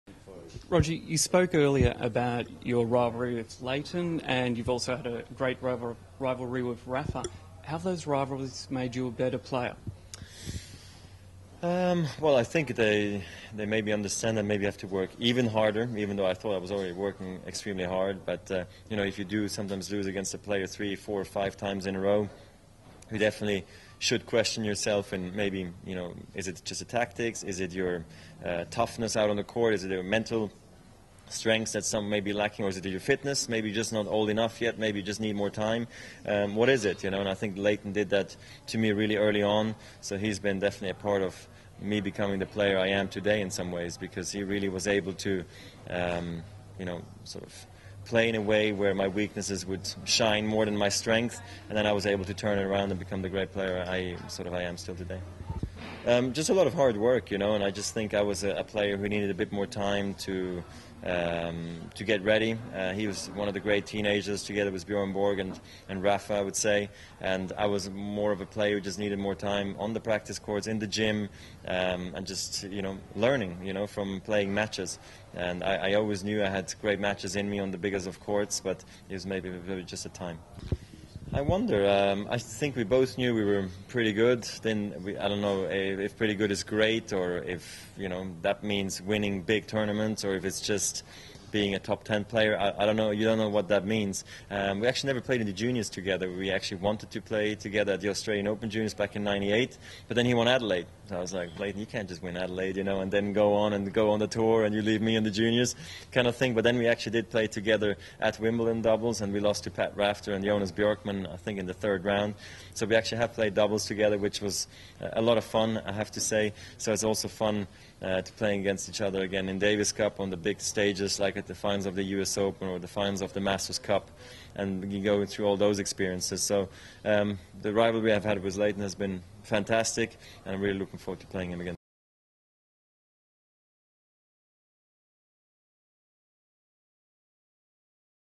Roger Federer on his rivalry with Lleyton Hewitt
Roger Federer speaks exclusively to tennis.com.au about his rivalry with former world No.1 Lleyton Hewitt.